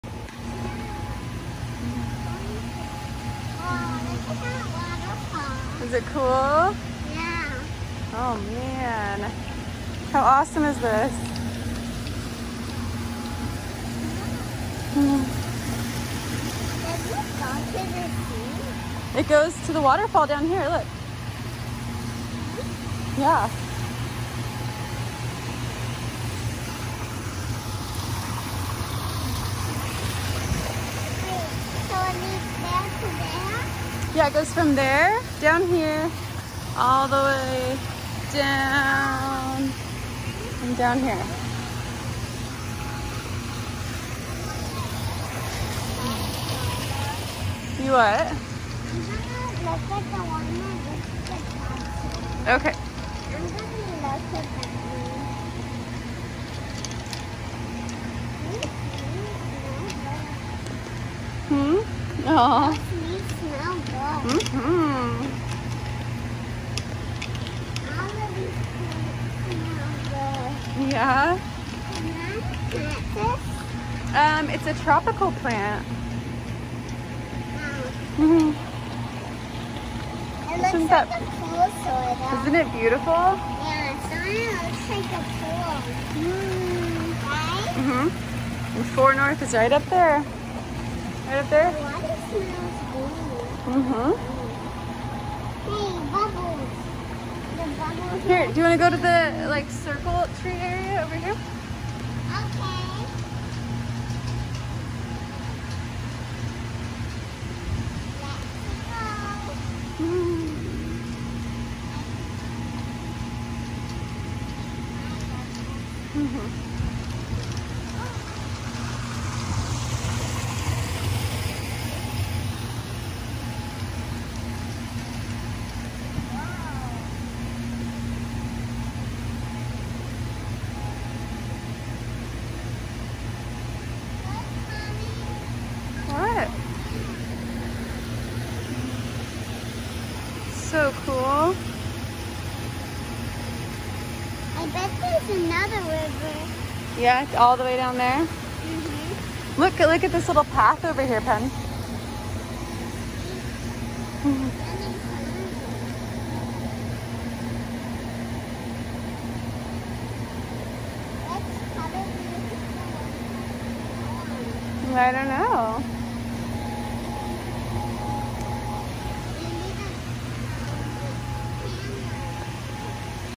Penn got to go to the healing garden at Dell Children’s Hospital for a bit.
And then got really sick later that night and is back to being confined to the room :(

5 years old